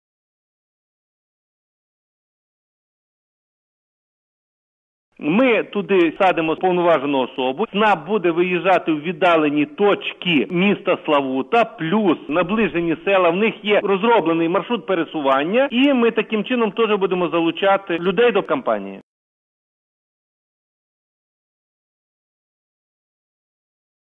Підписати декларацію з лікарями жителі Славутського району можуть у мобілльному ЦНАПі
Про це нам повідомив керівник Славутського центру первинної медико-санітарної допомоги Олег Гаврилюк. За його словами, таку послугу уже другий день надають у міському Центрі надання адмінпослуг. А з понеділка – надаватимуть у районному.